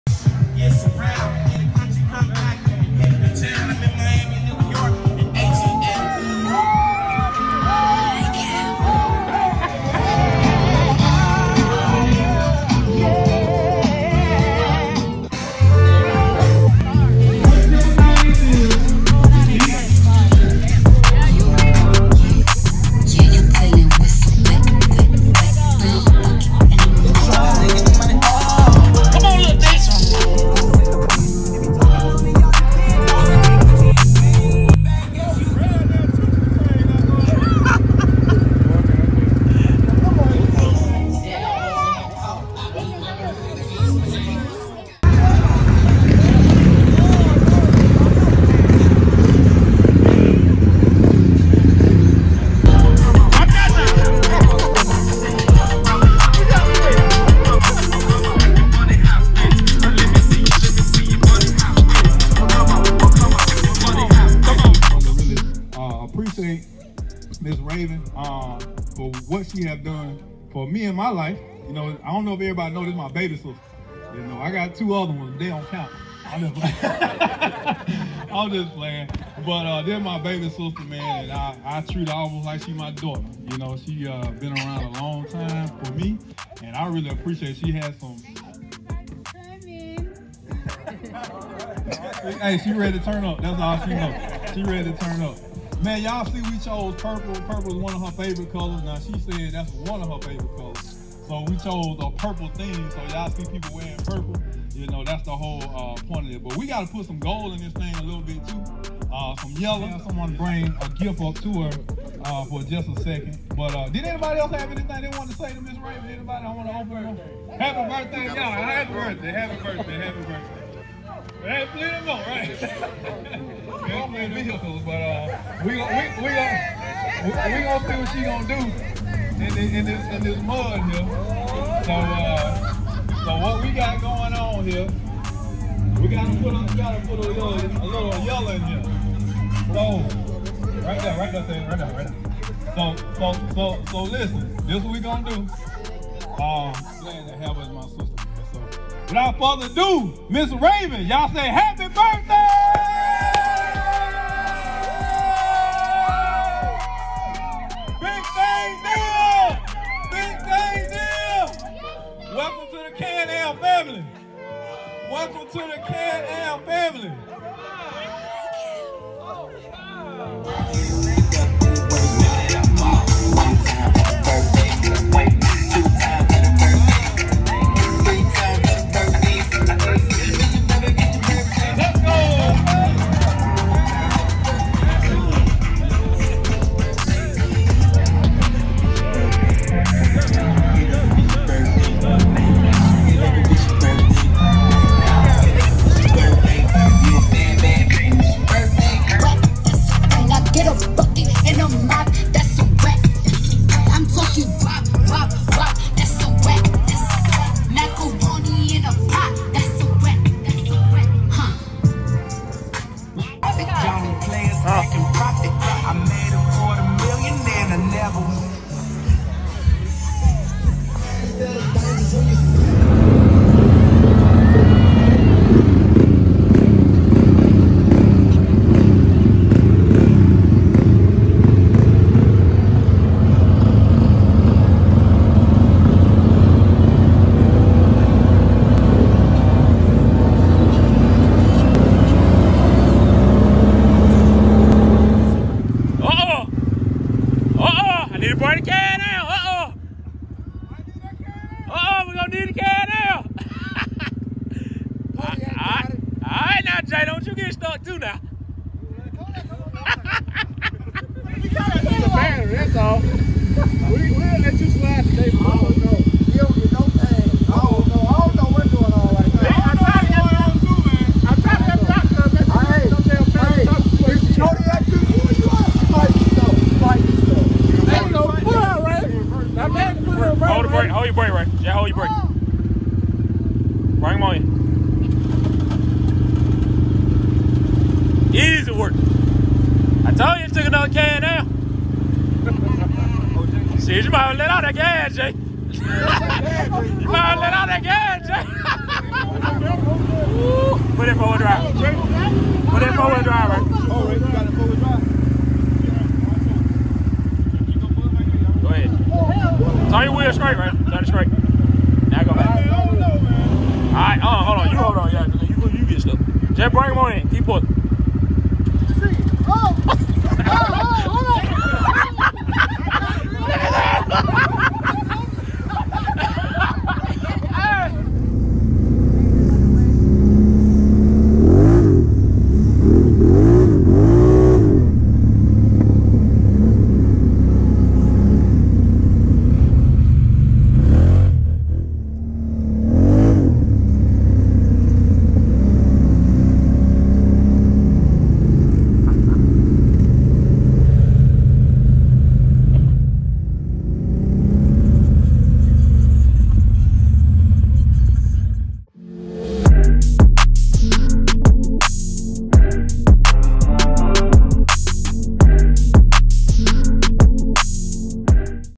Raven's Birthday | Bonfire | Trail Ride | Surprised With A Can Am Maverick XMR
Raven's Birthday | Bonfire | Trail Ride | Surprised Her With A Can Am Maverick XMR

Fire Water

Black Butter

Chronic Cleanse